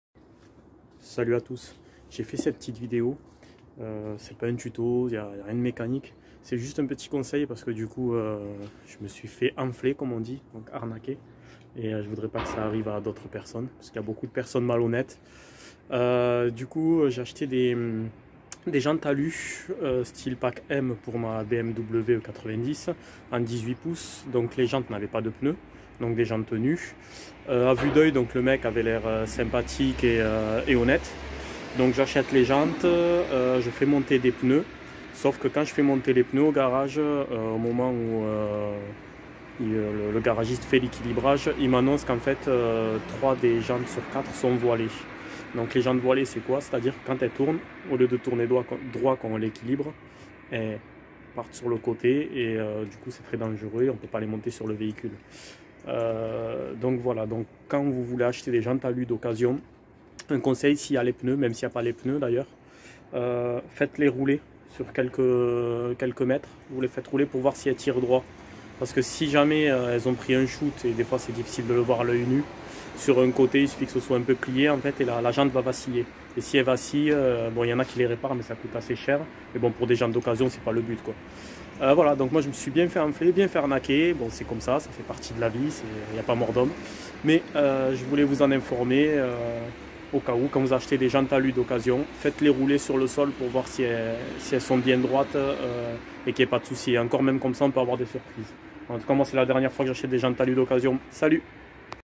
Jantes alu occasion Arnaque ? Ou Affaire ?
Les jantes d’occasion, une bonne idée ?

Niveau prix, pas de doute sur l’intérêt, mais attention à la qualité des produits. Prenons l’exemple d’un achat de jantes pour une BMW, jantes alu en 17 pouces. On va très facilement pouvoir trouver une offre large et intéressante sur internet mais une petite annonce retiendra forcément notre attention car une réduction supplémentaire, ça ne se refuse pas. Seulement voilà, même si l’on achète un produit d’occasion, une jante reste fragile. De multiples problèmes peuvent survenir : voilage, fissure, oxydation, plat, …